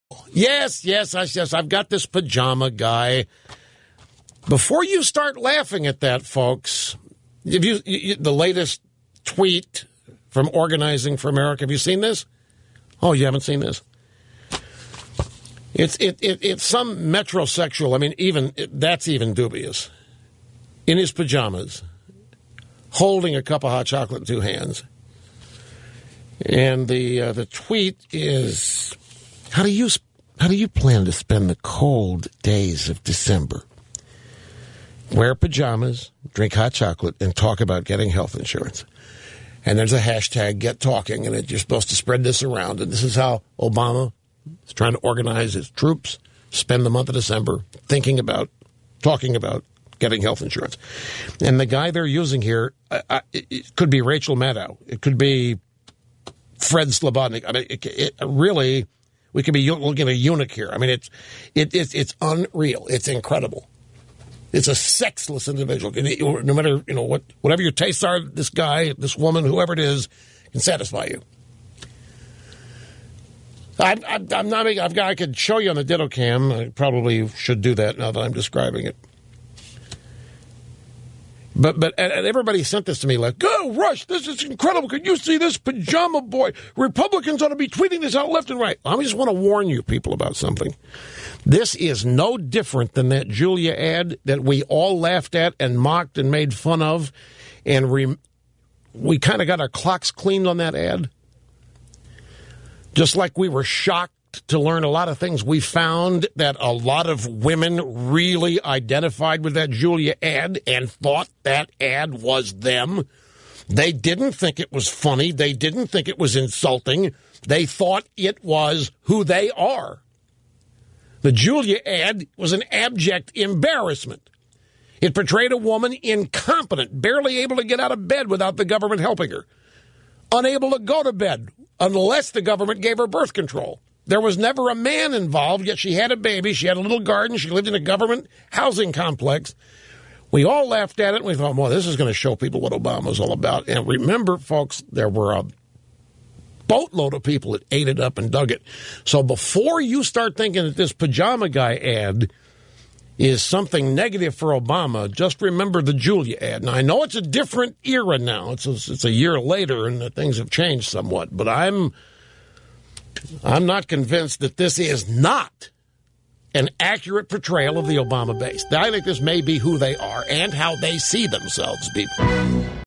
Obamacare-cheerleading Pajama Boy metrosexual(-or-beyond) is the new "Julia" (Rush Limbaugh)
Rush Limbaugh (12/18/13): "The guy they're using here could be Rachel Maddow. It could be Fred Slobodnik. Really, we could be looking at a eunuch here. It's unreal. It's incredible.  It's a sexless individual."